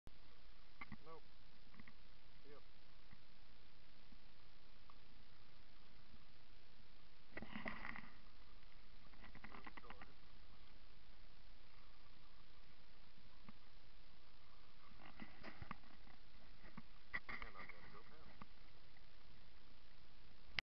Short canoe trip, 4-10-14
Trying the river for the first time this year. A little rougher than we wanted.